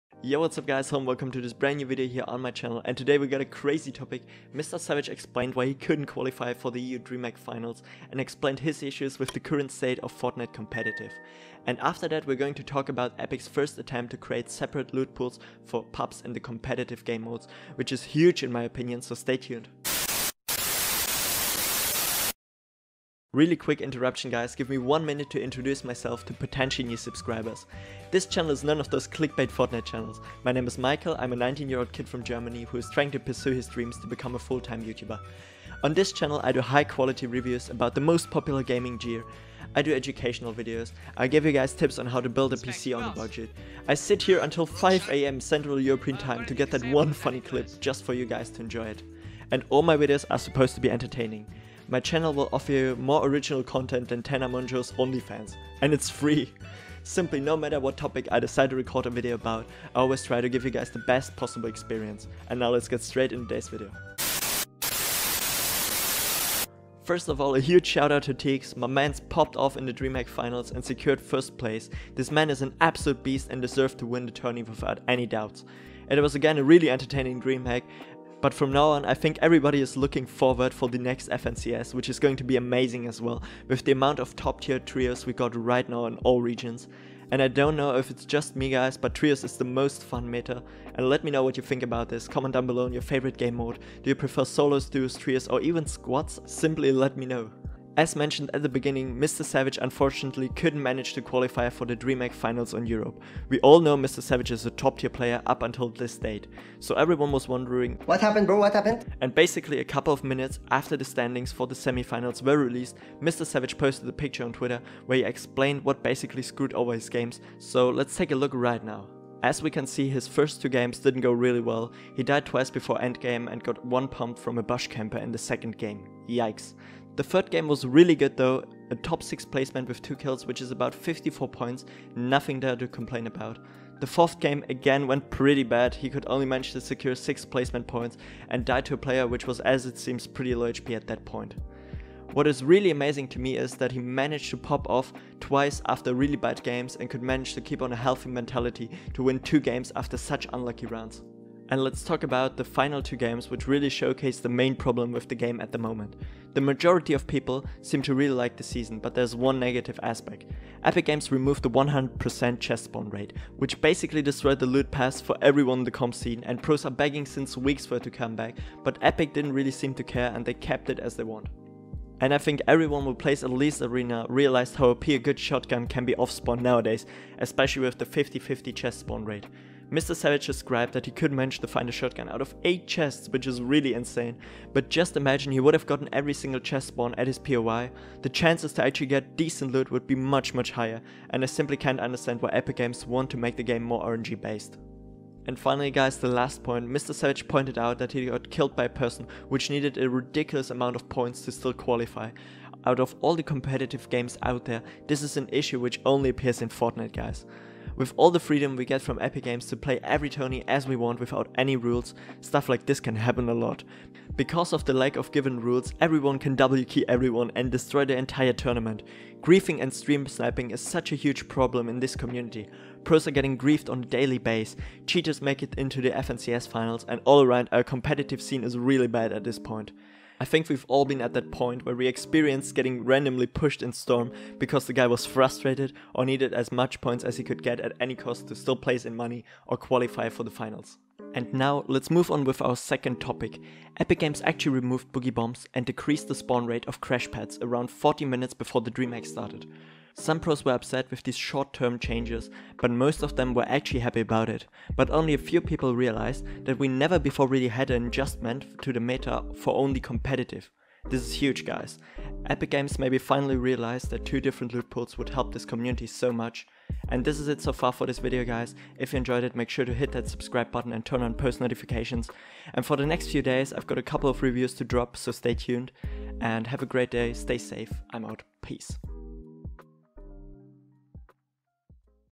MR SAVAGE *EXPLAINED* Why He Coud NOT Qualify for Dreamhack Finals... Epic Finally Separating .... ?
Mr Savage Tweeted About Why He Couldn´t Manage To Get Into Dreamhack Finals...
Epic Games Gave First Hints For *Separating Lootpools* In Fortnite!

I Finally Upgraded To *Adobe Premiere Pro 2020* So Expect The Best Possible Quality Just For You Guys .. You Are Truly Amazing!